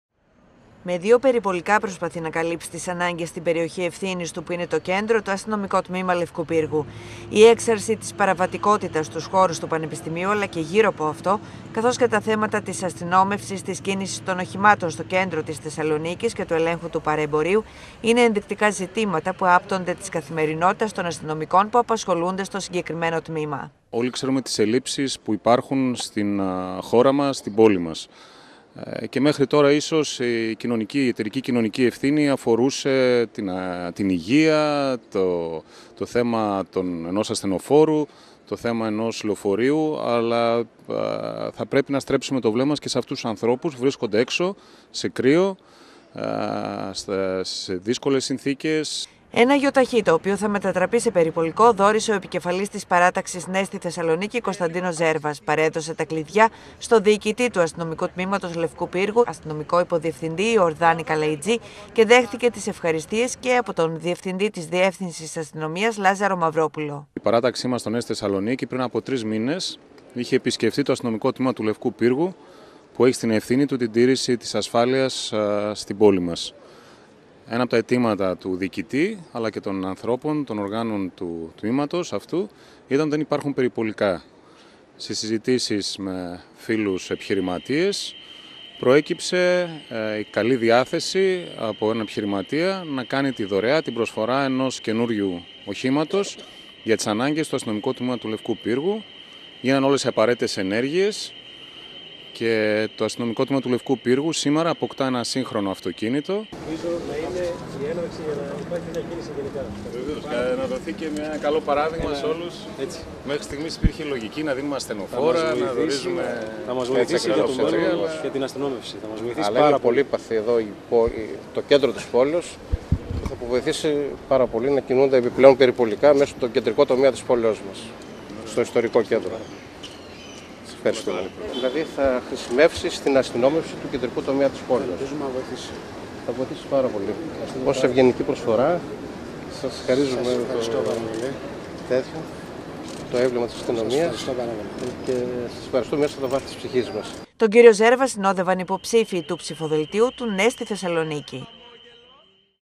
ΔΩΡΕΑ ΠΕΡΙΠΟΛΙΚΟΥ ΑΠΟ Κ. ΖΕΡΒΑ ΣΤΟ Α.Τ. ΛΕΥΚΟΥ ΠΥΡΓΟΥ (TV100-250219)
Ένα σύγχρονο Ι.Χ. το οποίο θα χρησιμοποιηθεί ως περιπολικό απέκτησε το Αστυνομικό Τμήμα Λευκού Πύργου, μετά από δωρεά επιχειρηματία. Πρόκειται για πρωτοβουλία του υποψηφίου δημάρχου Θεσσαλονίκης, Κωνσταντίνου Ζέρβα, ο οποίος προ ημερών είχε επισκεφθεί το Αστυνομικό Τμήμα και είχε ενημερωθεί για τις ελλείψεις που αντιμετωπίζει σε περιπολικά και υποδομές. Ο συνδυασμός «Ναι στη Θεσσαλονίκη» προχώρησε σε συζητήσεις με επιχειρηματίες και εν τέλει βρέθηκε ένας ο οποίος αποφάσισε να προχωρήσει στη δωρεά του οχήματος.
Ρεπορτάζ: Αντιγόνη Σφυρή
Κάμερα: Γιώργος Βογιατζόγλου
Μοντάζ: Παντελής Ταχτσής
Μιλούν: Κωνσταντίνος Ζέρβας (Επικεφαλής “Ναι στη Θεσσαλονίκη”),
Λάζαρος Μαυρόπουλος (Διευθυντής Διεύθυνσης Αστυνομίας Θεσσαλονίκης)